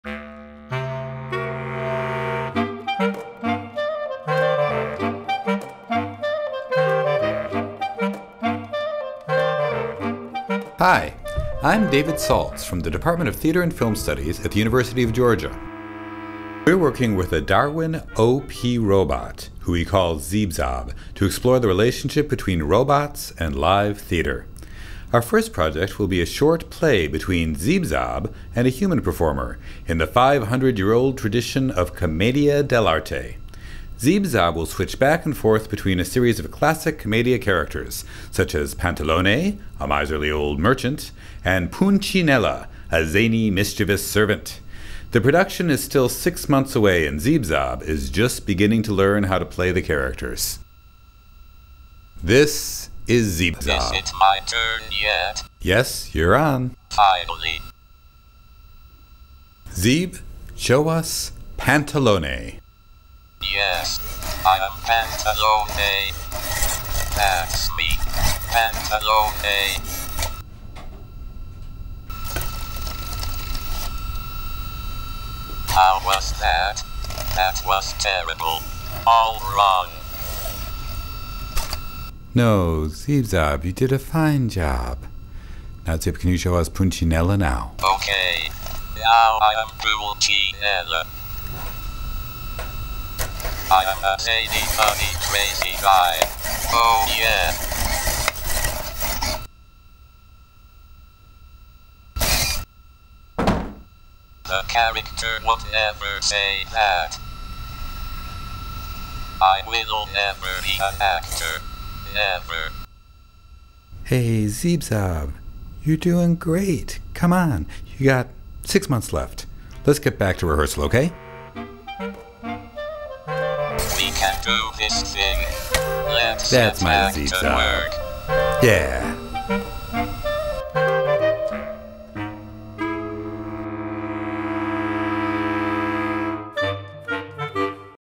UGA Theatre and Film's Zeeb Zob the Robot Learns Commedia dell'Arte
This little robot will soon perform in the style of Italian Commedia dell'Arte. The project is centered in the University of Georgia Department of Theatre and Film Studies, under the direction of Dr. David Z. Saltz, in collaboration with Dr. Chi Thai in the Department of Biological and Agricultural Engineering. The robot is a DARwin-OP, manufactured by Robotis. It is being programmed in Max/MSP, using the DarwinAnimator tool developed by David Saltz in conjunction with TellDarwin, developed by Michael Krzyzaniak. This project is supported by Ideas for Creative Exploration (ICE) and the UGA Center for Teaching and Learning.